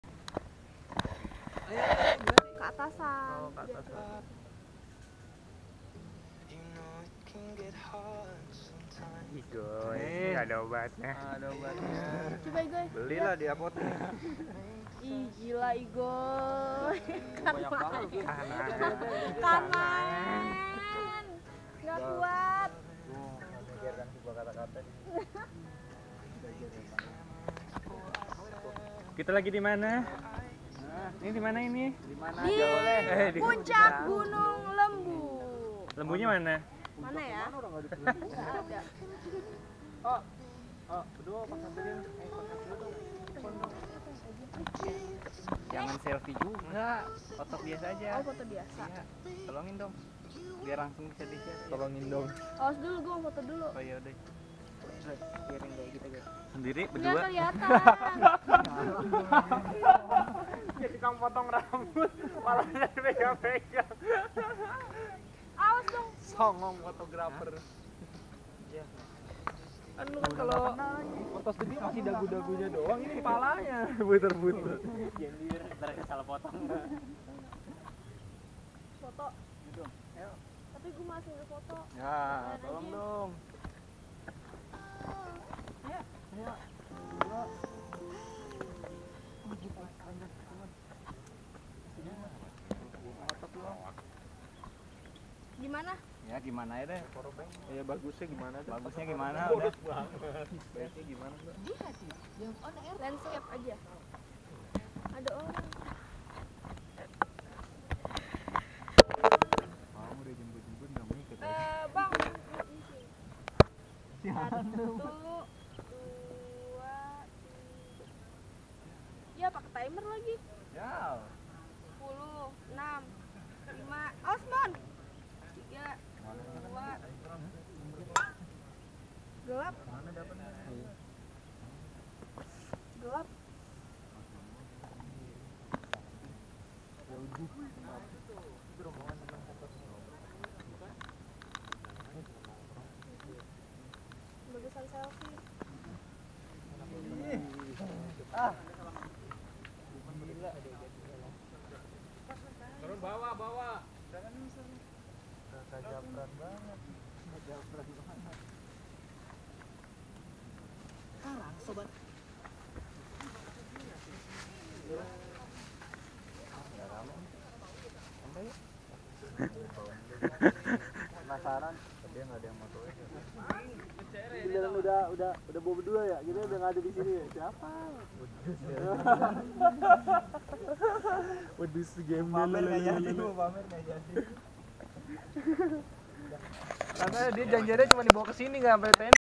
Wefie di objek Wisata Batu Besar, Gunung Lembu, Jatiluhur, Purwakarta, Jawa Barat
I am traveller and sometimes I take video based on what happened and I present for beloved viewer and audience, for public consumptions.